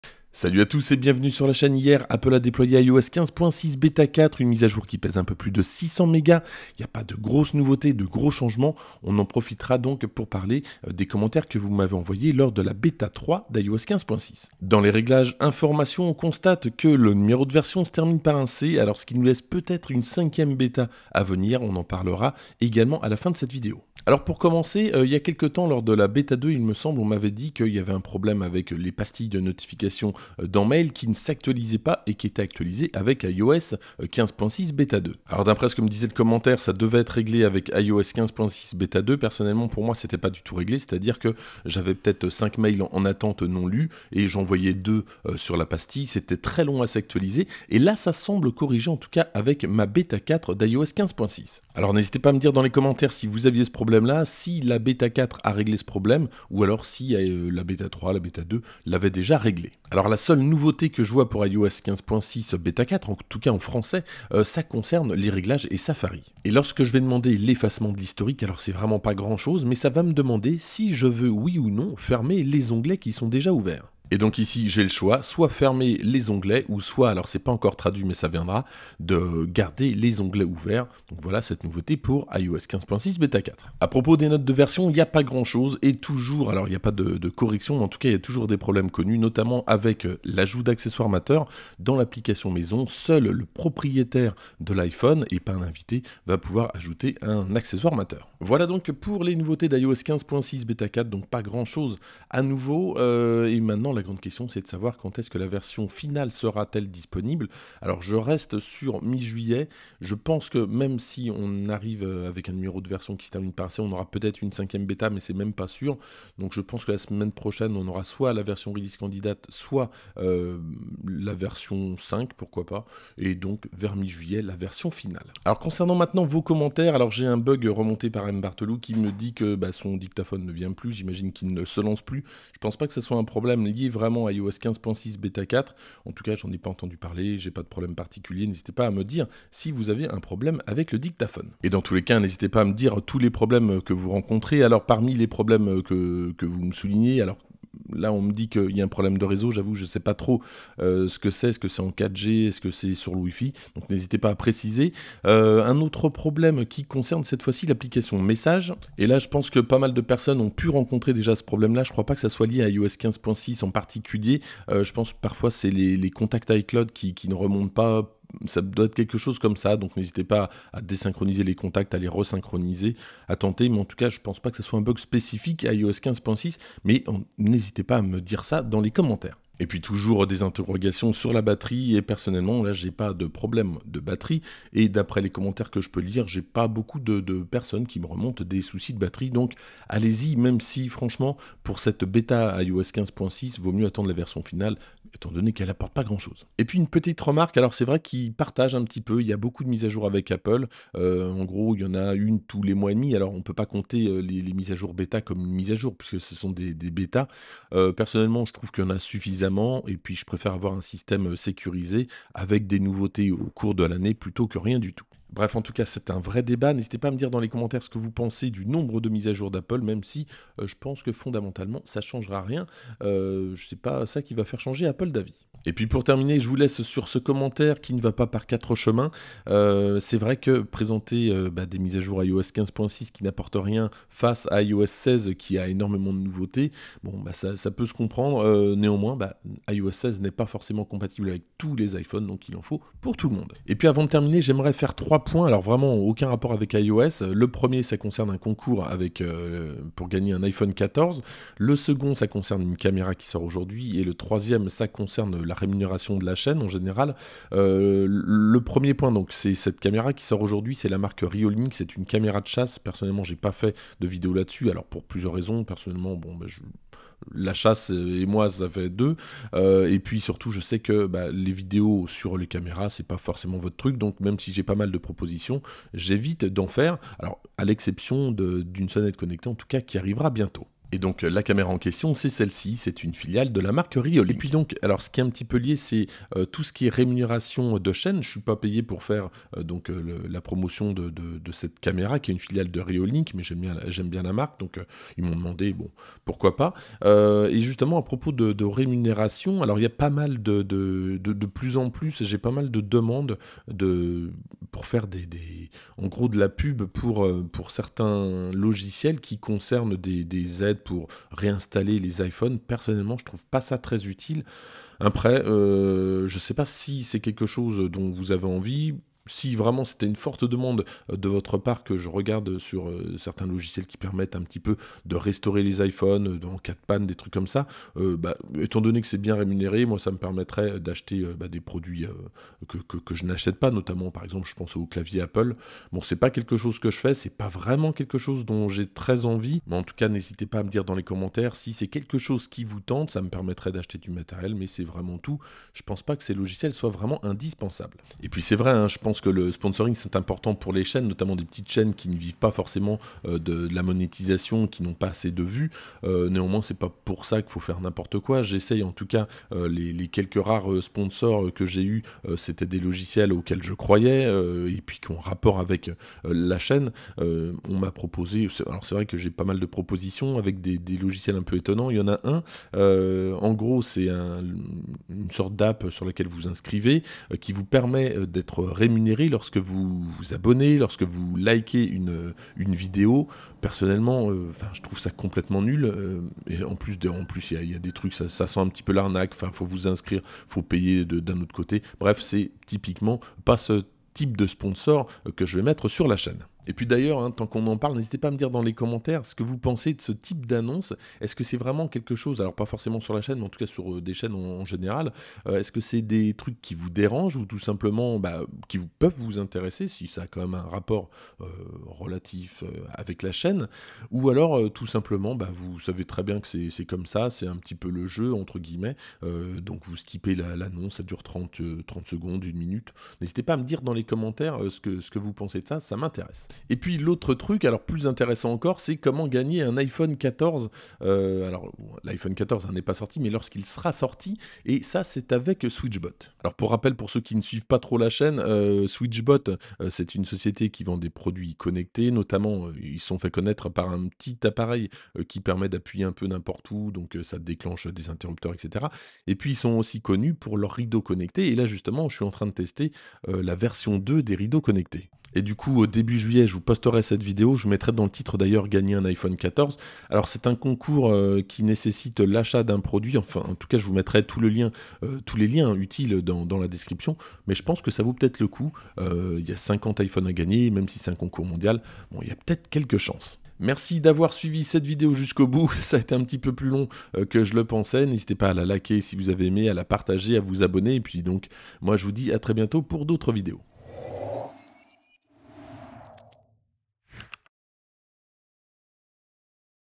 iOS 15.6 beta 4 : Nouveautés sur iPhone + Vos commentaires de la beta 3
Apple a mis à disposition iOS 15.6 beta 4, on discute des nouveautés dans cette vidéo.
REOLINK sur Amazon